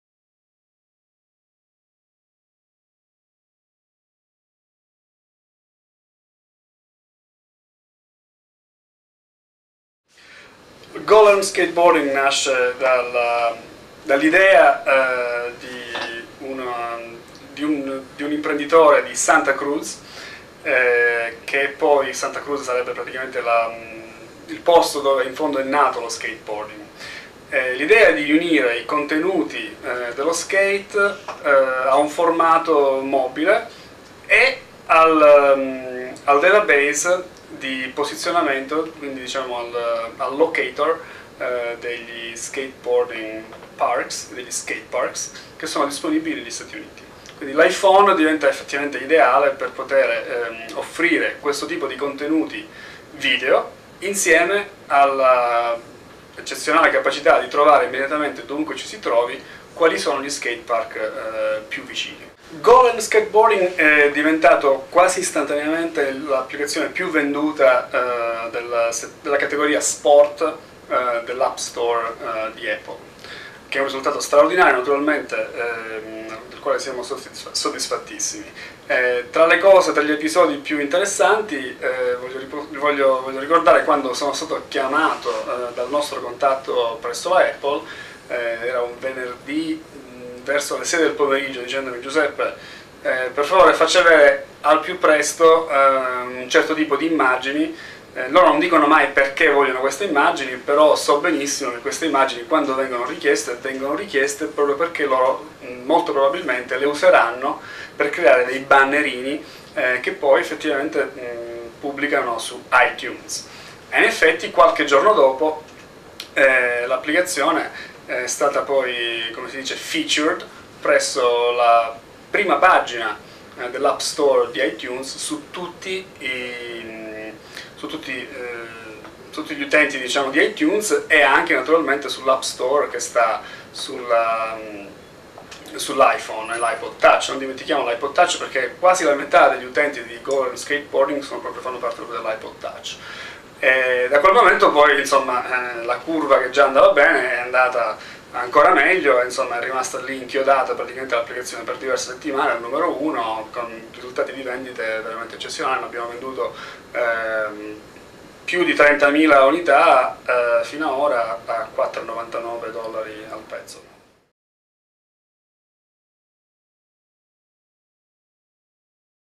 Wave Factory intervista Giuseppe Taibi (1 di 5)
Prima parte dell'intervista a Giuseppe Taibi, fondatore e CEO di SmartWorlds Inc, realizzata da Wave Factory.